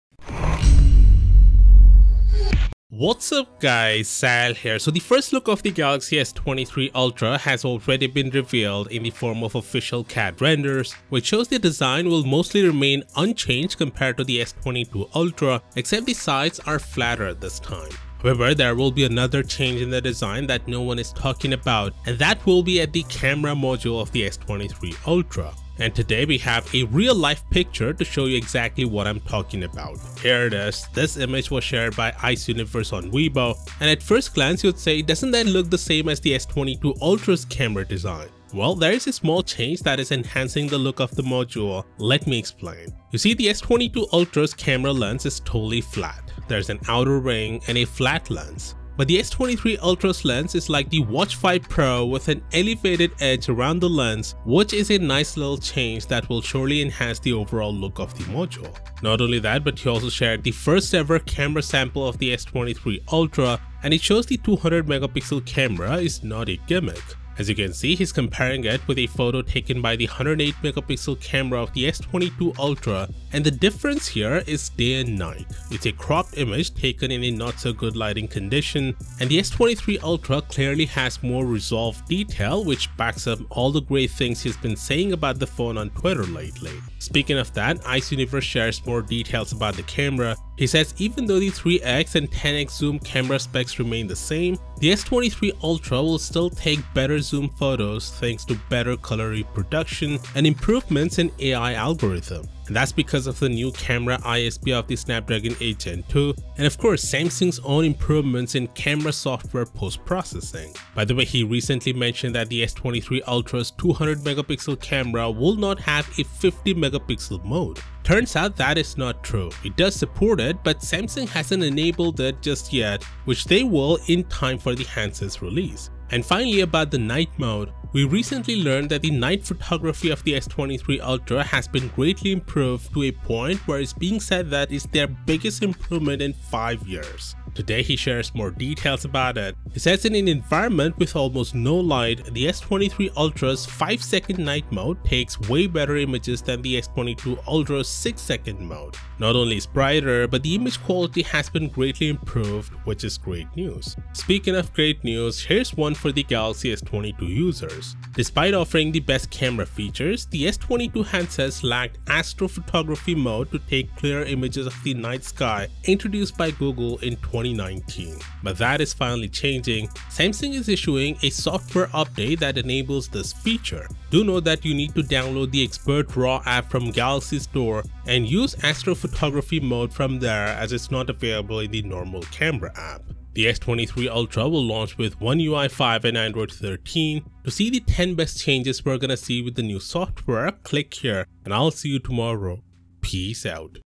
Samsung Galaxy S23 Ultra - First REAL Look
Samsung Galaxy S23 Ultra will have a design that resembles its predecessor but there will be some changes and one of them is shown in the video. Samsung Galaxy S23 Ultra will have a camera module that takes design cues from the Watch 5 Pro. We also have the camera samples of the Galaxy S23 Ultra that show dramatic improvements. Samsung will launch the Galaxy S23 Ultra at the Unpacked Event in January.